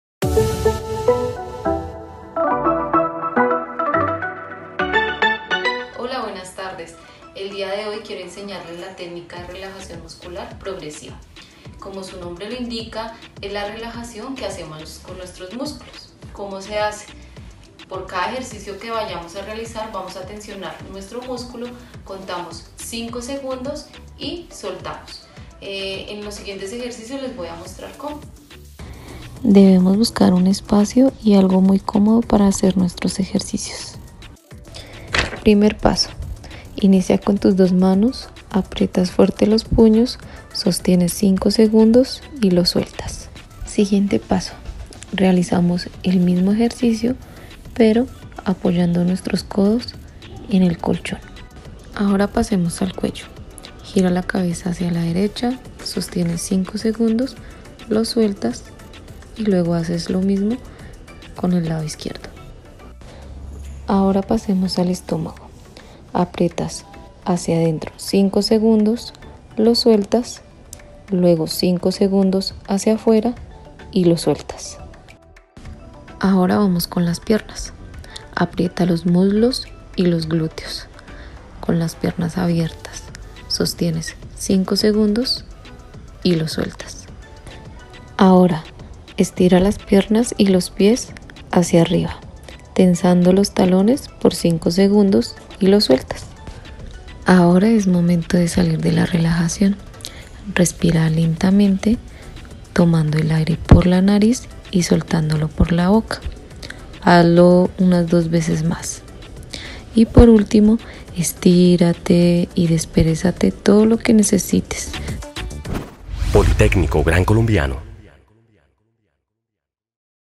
Relajación Muscular Progresiva
En un paso a paso sabrás tips de relajación muscular progresiva.